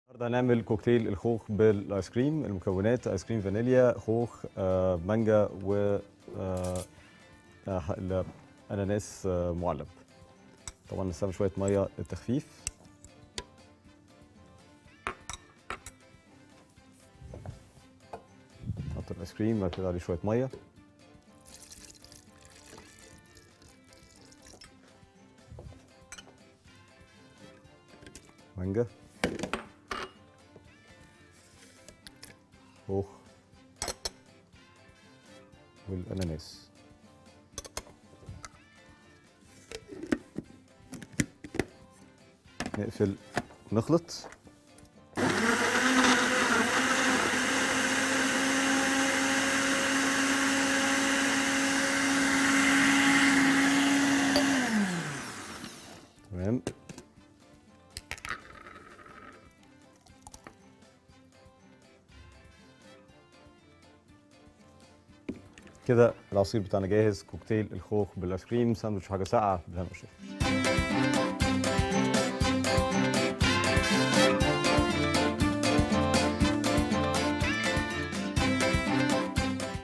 كوكتيل الخوخ بالاناناس | شريف الحطيبي
طريقة عمل كوكتيل الخوخ بالاناناس  مع شريف الحطيبي في سندوتش وحاجة ساقعة